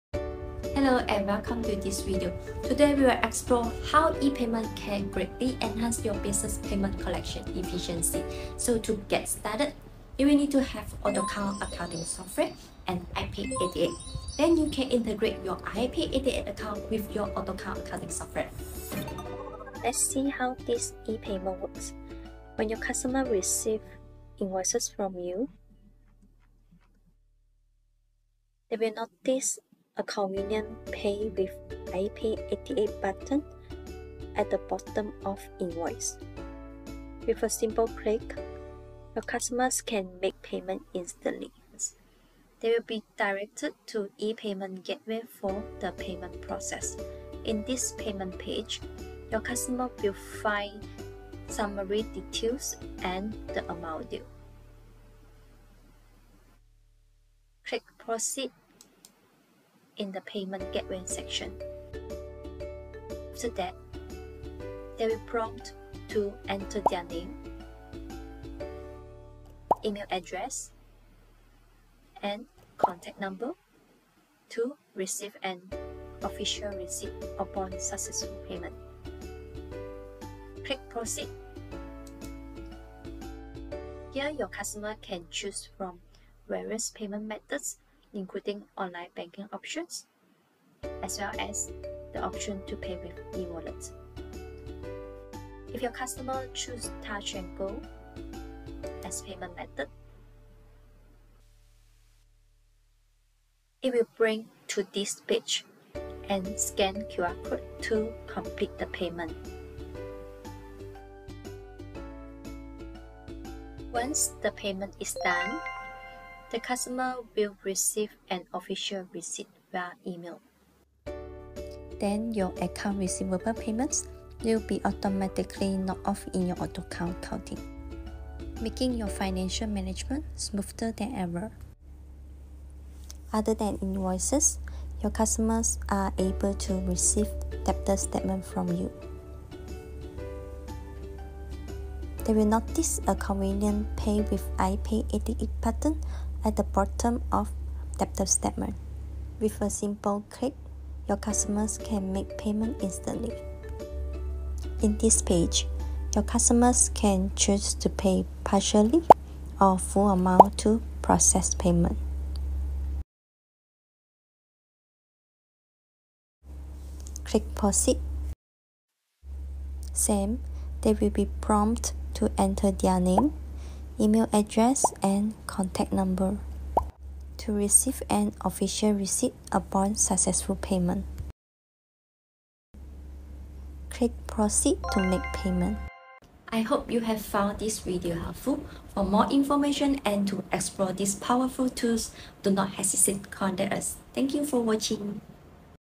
AutoCount Accounting E-Payment Plugin (English Version)
Autocount x iPay88
Invoicing solution with integrated payments
Smarter way to accelerate your customer payment collection

Swift invoicing
- Create digital invoices and share them with your customers via e-mail, messaging apps or SMS.

Automate your process
- Streamline and automate the entire invoicing process to significantly reduce manual tasks and administrative burdens.

- Users can quickly input essential invoice details, such as client information, products or services rendered, and payment terms.

Seamless compatibilty
- Users can effortlessly sync invoicing data with existing business infrastructure and work harmoniously with AutoCount Accounting.

Data centric management
- Achieve a more unified and holistic approach in managing finances, enhancing productivity, and gaining valuable insights from consolidated data sources.

More payment options
- Accept a wide range of online payment methods such as online banking, e-wallet, BNPL, and bank cards.

Enhance customer satisfaction
- Empower clients to choose the method that suits them best, increasing customers’ sense of control and flexibility.

Be a market leader
- Create a favorable impression of professionalism and efficiency among clients, leading to a stronger reputation in the market. Satisfied customers are more likely to engage in repeat business.

Auto knock-off
- Reduce workload as auto knock-off in AutoCount accounting after payment made.